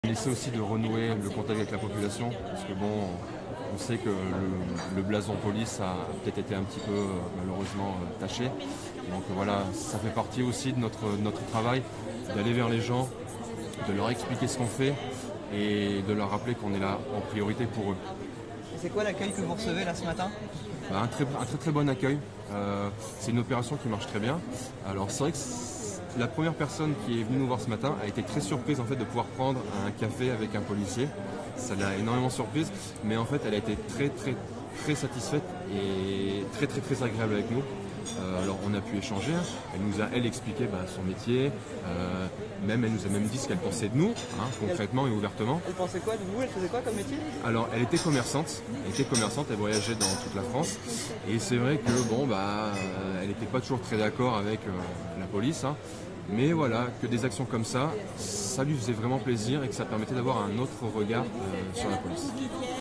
ITW un cafe avec un poilicer
Un café avec un policier est une opération organisée par les commissariats de Brive-la-Gaillarde et de Tulle ce mercredi 9 octobre 2019. Un rendez-vous privilégié pour permettre d’aborder avec un policier des questions liées à la sécurité, au recrutement, aux démarches administratives, aux problèmes rencontrés.
Equipe : Mathilde Brazeau et Laurence Couvrand
Intervenant :
Stephen – Adjoint de sécurité au commissariat de Brive